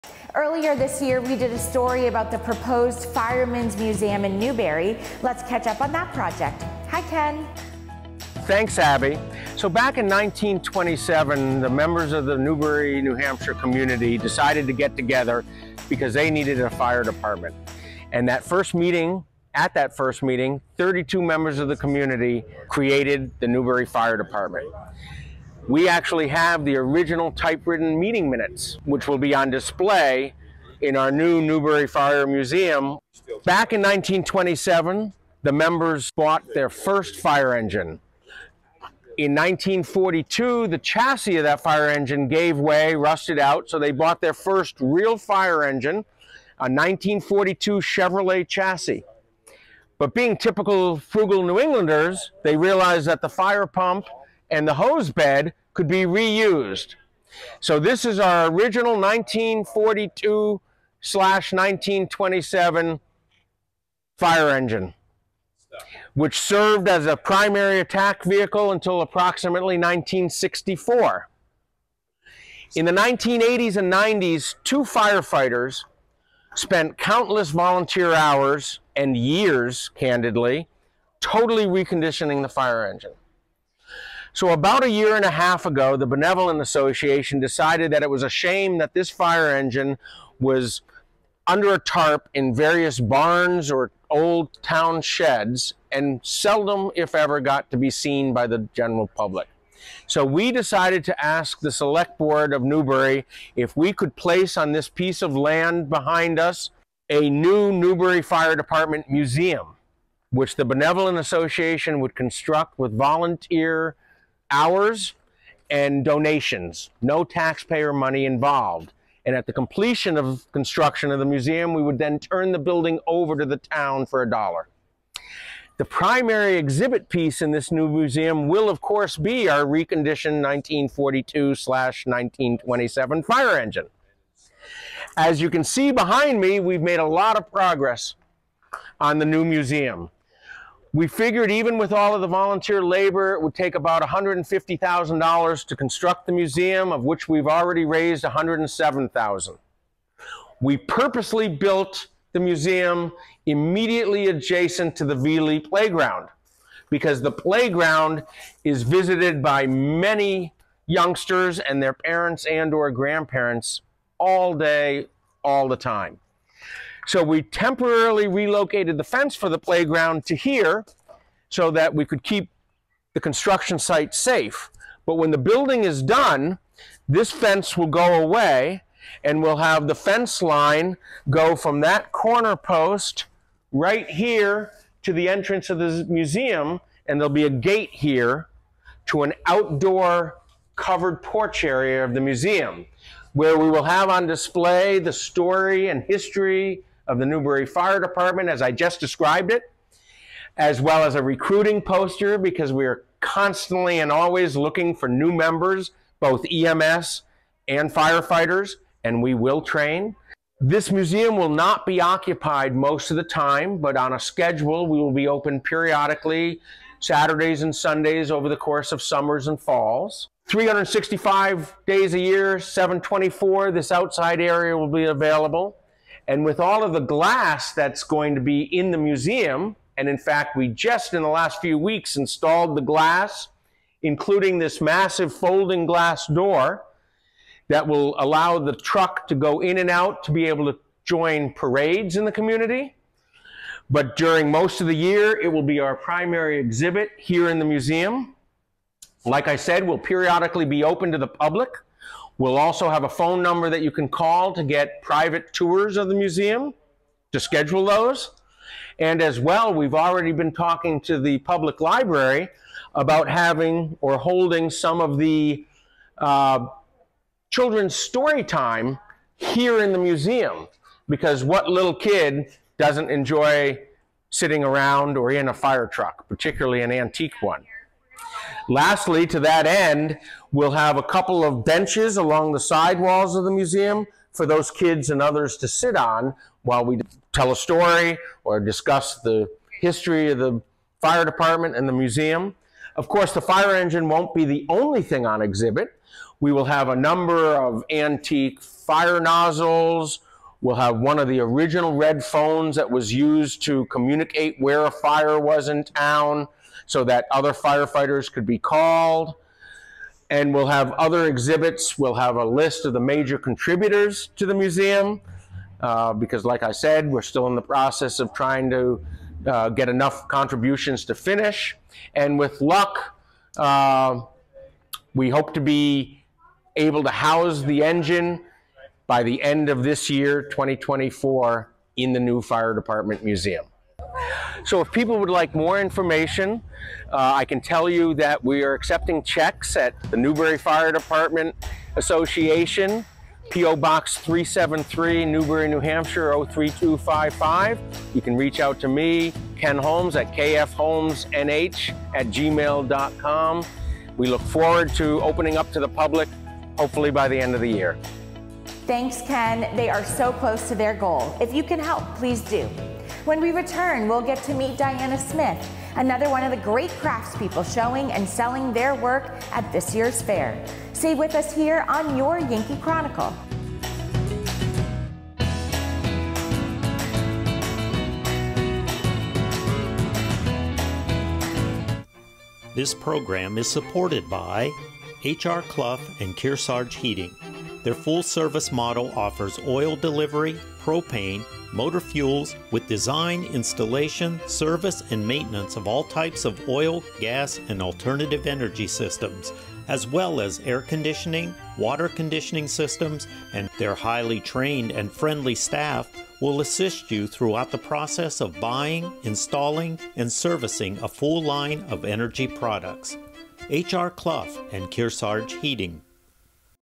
Newbury Firefighters Museum UPDATE
Ken Holmes will give us an update on progress at the Newbury Fireman’s Museum.